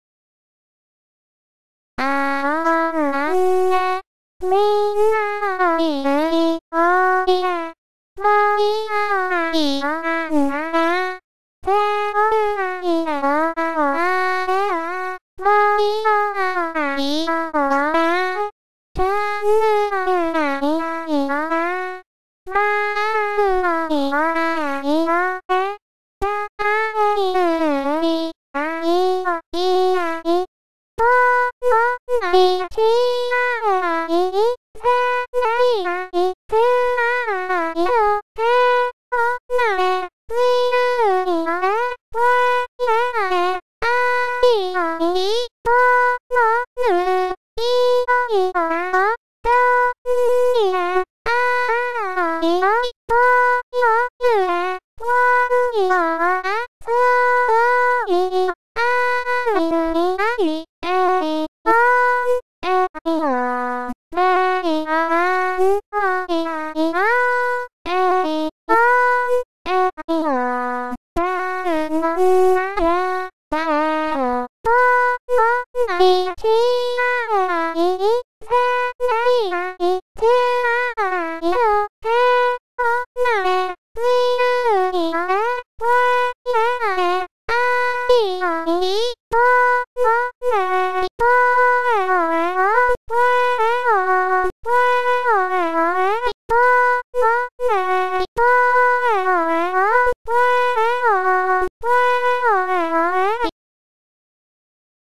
{UTAU} Murasaki Yuni-Tuning test
I need help tuning her because, as you can hear, I kinda failed. If you choose to help tune her the download link is
I did not upload her after I attempted to tune her so you'll be starting from scratch. If you like her voice you may use her for your own songs. Thanks.